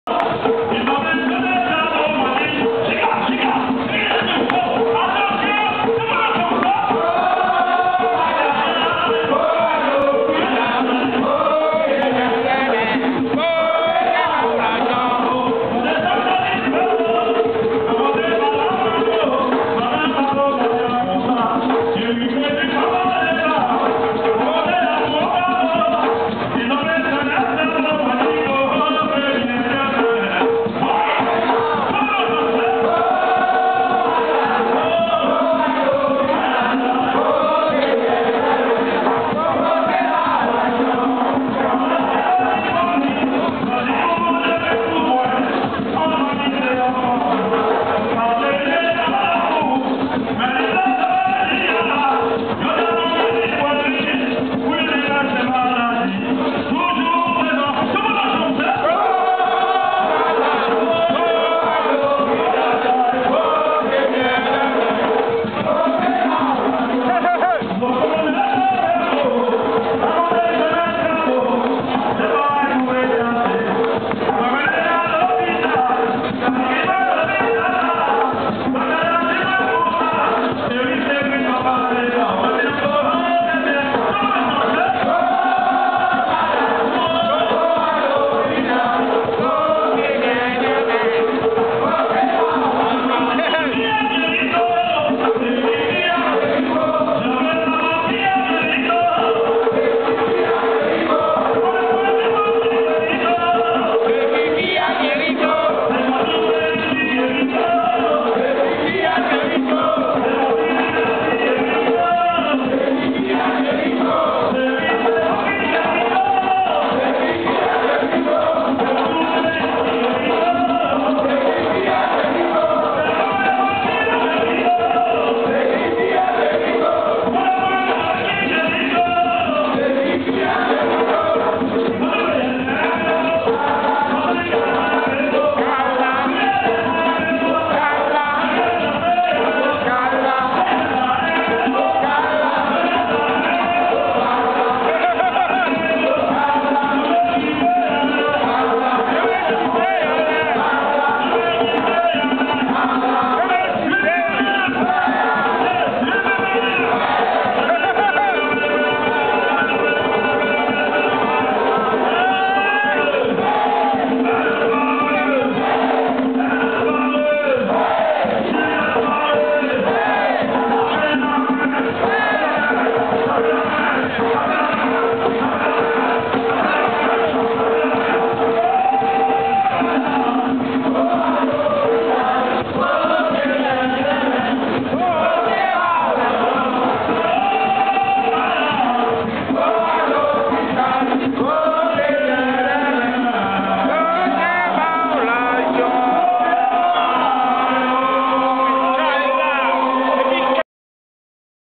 ERIC NEGRIT-HOMMAGE A TI RAOUL AU CHAPITEAU DE SARCELLES
Eric Negrit rend hommage à Ti Raoul au chapiteau de Sarcelles.